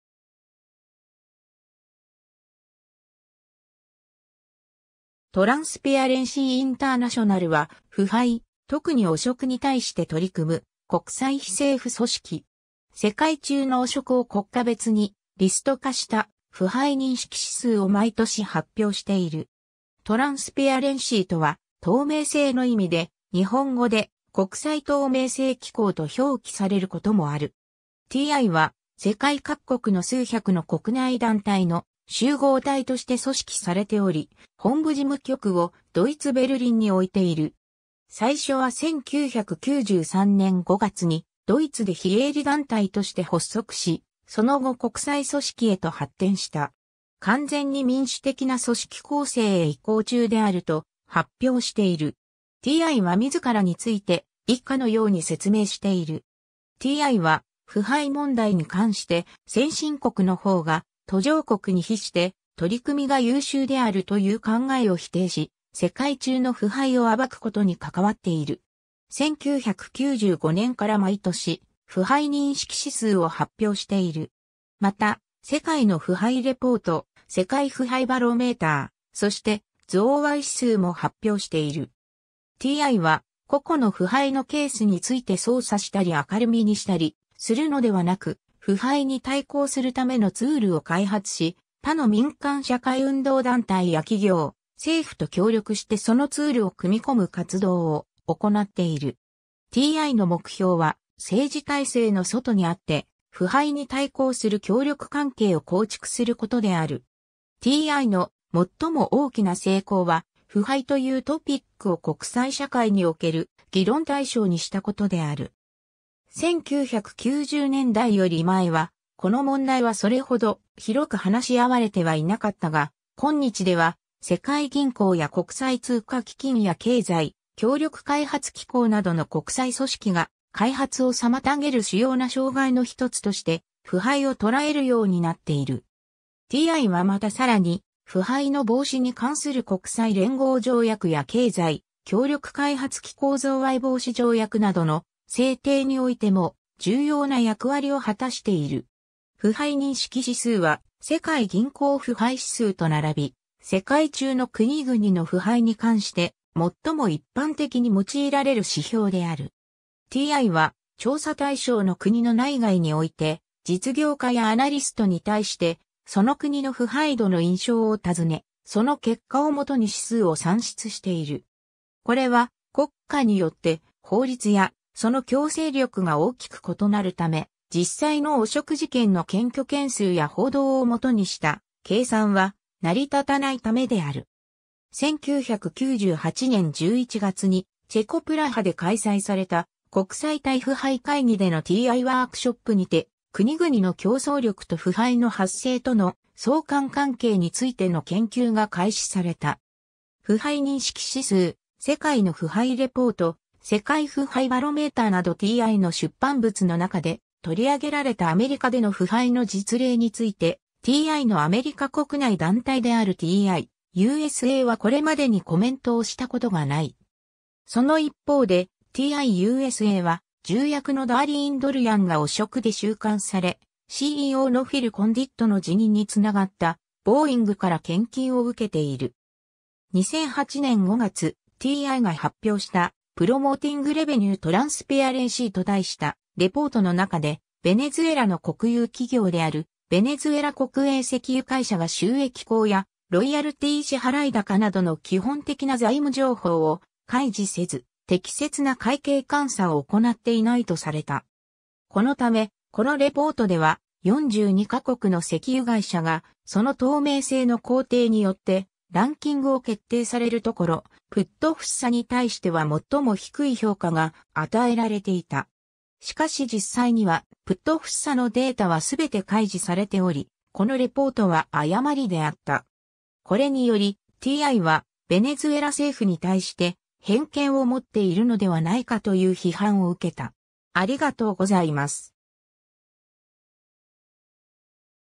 トランスペアレンシー・インターナショナル
トランスペアレンシー・インターナショナル（英語: Transparency International, 略称：TI）は、腐敗、特に汚職に対して取り組む国際非政府組織。
世界中の汚職を国家別にリスト化した「腐敗認識指数」を毎年発表している。
トランスペアレンシーとは「透明性」の意味で、日本語で国際透明性機構と表記されることもある。
 TI は、世界各国の数百の国内団体の集合体として組織されており、本部事務局をドイツ・ベルリンに置いている。
最初は1993年5月にドイツで非営利団体として発足し、その後国際組織へと発展した。
完全に民主的な組織構成へ移行中であると発表している。
 TI は自らについて以下のように説明している。
 TI は腐敗問題に関して「先進国のほうが途上国に比して取り組みが優秀である」という考えを否定し、世界中の腐敗を暴くことに関わっている。
 1995年から毎年「腐敗認識指数」を発表している。
また、「世界の腐敗レポート」(Global Corruption Report)、「世界腐敗バロメーター」(Global Corruption Barometer)、そして「贈賄指数」(Bribe Payers Index) も発表している。
 TI は個々の腐敗のケースについて捜査したり明るみにしたりするのではなく、腐敗に対抗するためのツールを開発し、他の民間社会運動団体や企業、政府と協力してそのツールを組み込む活動を行っている。
TI の目標は、政治体制の外にあって腐敗に対抗する協力関係を構築することである。
 TI の最も大きな成功は、腐敗というトピックを国際社会における議論対象にしたことである。
1990年代より前は、この問題はそれほど広く話し合われてはいなかったが、今日では世界銀行や国際通貨基金や経済協力開発機構などの国際組織が、開発を妨げる主要な障害のひとつとして腐敗を捉えるようになっている。
TI はまたさらに、腐敗の防止に関する国際連合条約や 経済協力開発機構贈賄防止条約 (OECD Anti-Bribery Convention) などの制定においても、重要な役割を果たしている。
 腐敗認識指数は、世界銀行腐敗指数 (World Bank corruption index) と並び、世界中の国々の腐敗に関して、最も一般的に用いられる指標である。
TI は調査対象の国の内外において、実業家やアナリストに対して、その国の腐敗度の印象を尋ね、その結果を基に指数を算出している。
これは、国家によって法律やその強制力が大きく異なるため、実際の汚職事件の検挙件数や報道を基にした計算は、成り立たないためである。
 1998年11月にチェコ・プラハで開催された、国際対腐敗会議 (International Anti-Corruption Conference) での TI ワークショップにて、国々の競争力と腐敗の発生との相関関係についての研究が開始された。
 腐敗認識指数、世界の腐敗レポート、世界腐敗バロメーターなど TI の出版物の中で取り上げられたアメリカでの腐敗の実例について、TI のアメリカ国内団体である TI-USA はこれまでにコメントをしたことがない。
その一方で、TI-USA は重役のダーリーン・ドルヤンが汚職で収監され、CEO のフィル・コンディットの辞任につながったボーイングから献金を受けている。
 2008年5月、TI が発表した「Promoting Revenue Transparency」と題したレポートの中で、ベネズエラの国有企業であるベネズエラ国営石油会社 (PDVSA) が収益高やロイヤルティー支払高などの基本的な財務情報を開示せず、適切な会計監査を行なっていないとされた。
このため、このレポートでは、42 か国の石油会社がその透明性の高低によってランキングを決定されるところ、PDVSA に対しては最も低い評価が与えられていた。
しかし実際には、PDVSA のデータはすべて開示されており、このレポートは誤りであった。
これにより、TI はベネズエラ政府に対して偏見を持っているのではないかという批判を受けた。